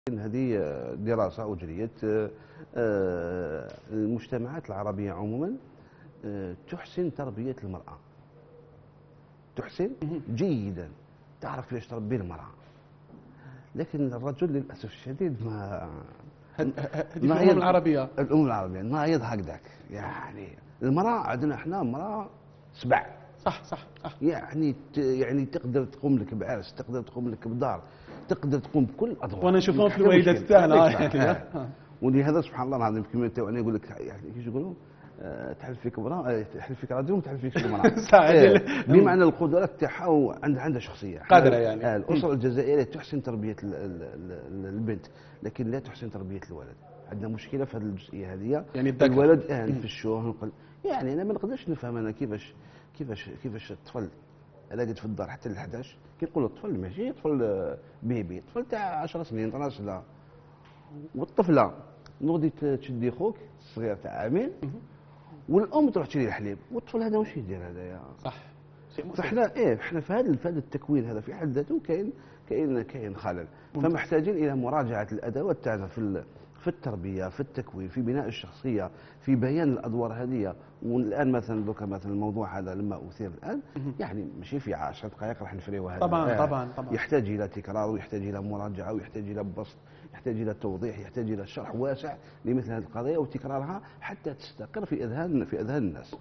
الشيخ عبد القادر حموية.."المجتمعات العربية عموما تُحسن تربية الفتاة ..لكن لا تحسن تربية الولد "
HD 10992/5000
SD 10921/27500/V